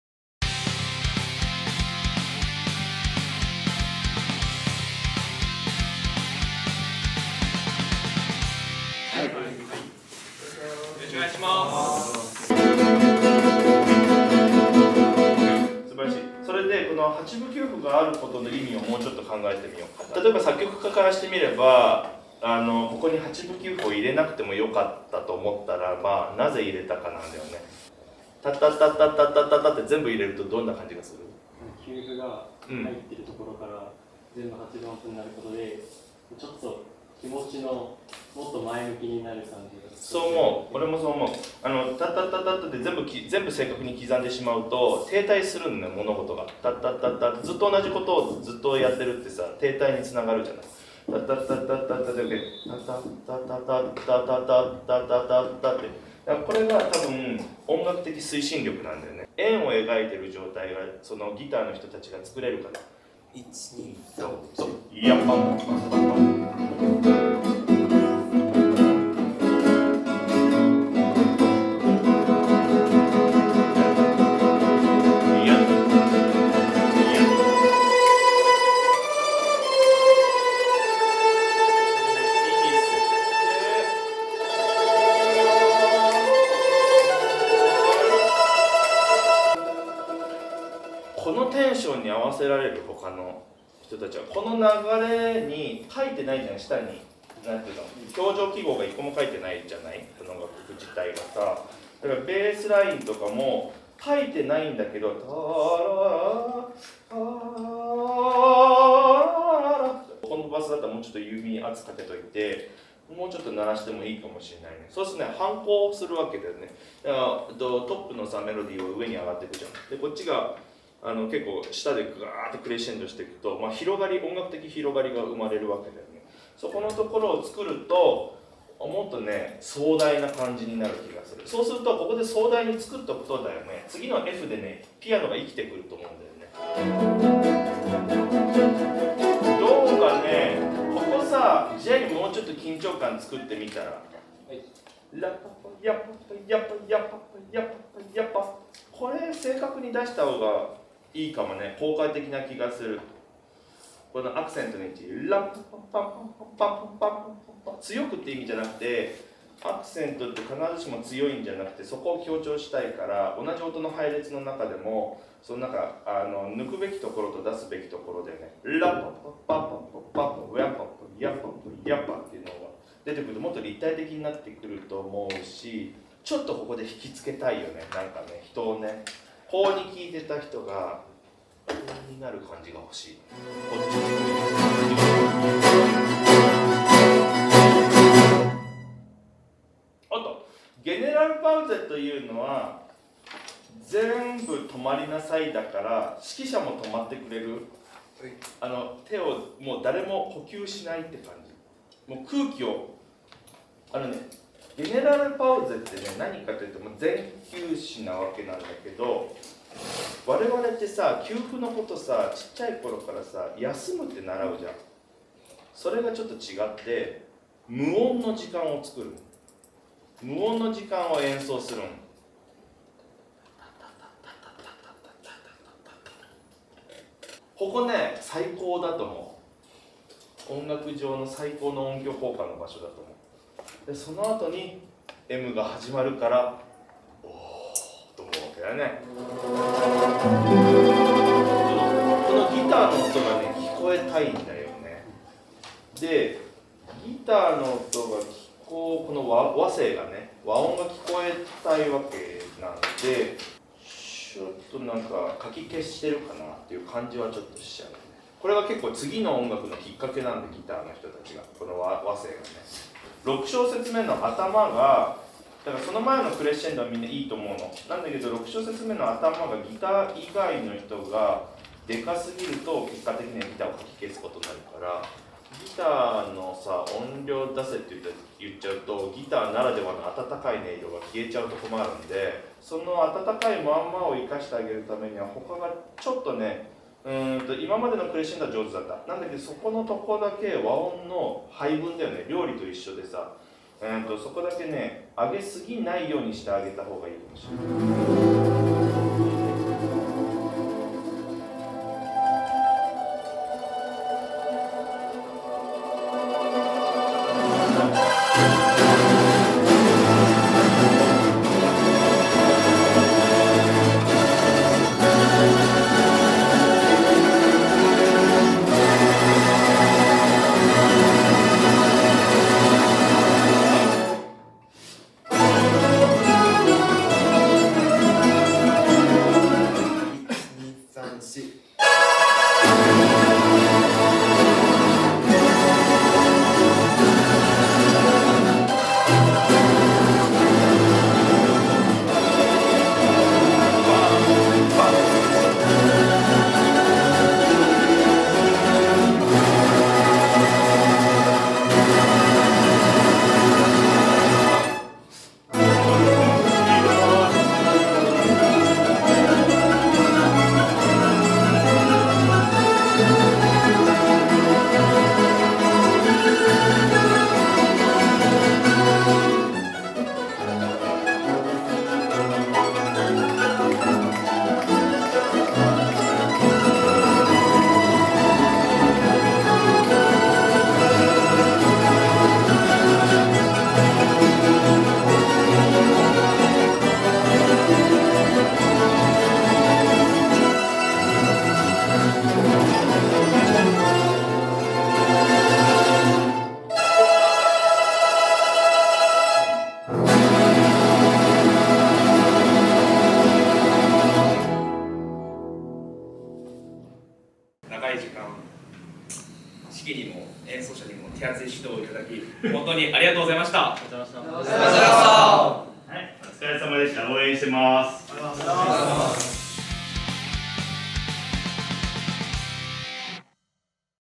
【令和5年度次世代育成文化部活動応援事業】ギター・マンドリン部【群馬県立前橋高等学校】
【次世代育成文化部活動応援事業とは？】

 指導者の確保が難しい県内の中学・高校の文化部でも、よりレベルの高いプロによる指導を受けられるように、
当事業団が群馬県内で活動しているアーティストとのマッチングを行い、各校の要望に応じた講演・実技指導を実施する事業です。